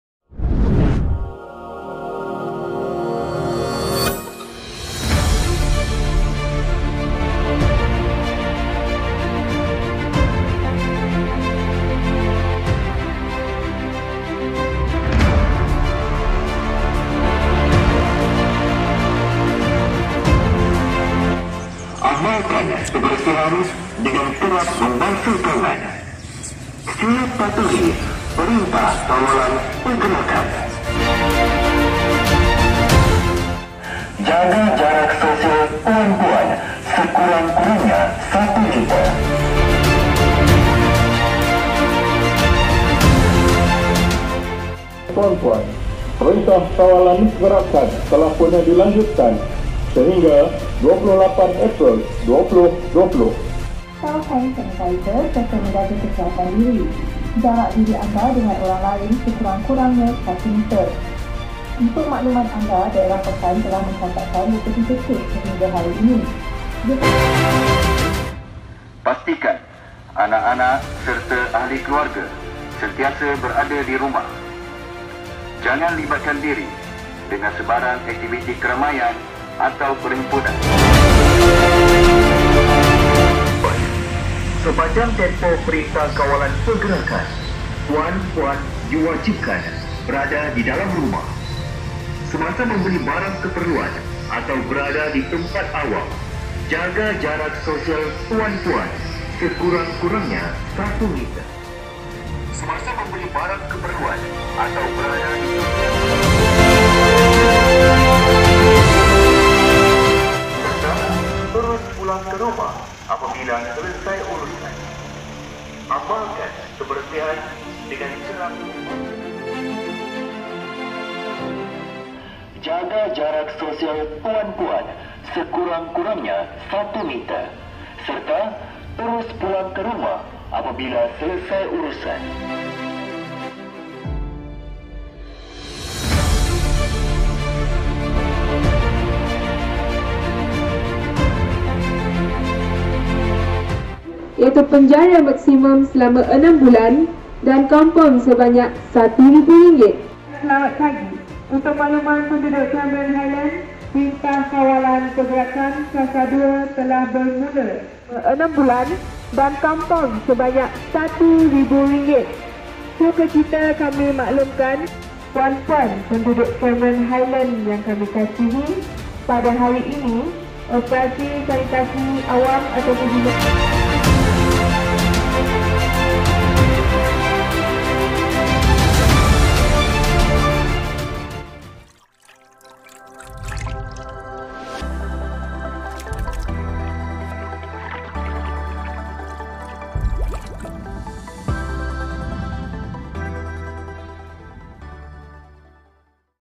INFO ON WHEELS : Rangkuman IOW JAPEN Pahang 13 April 2020.
Sekitar aktiviti Info On Wheels Jabatan Penerangan Negeri Pahang berkaitan Perintah Kawalan Pergerakan di seluruh negara bermula dari 18hb Mac hingga 28hb April 2020.

'Anda bantu kami, duduk di rumah, biar kami bekerja'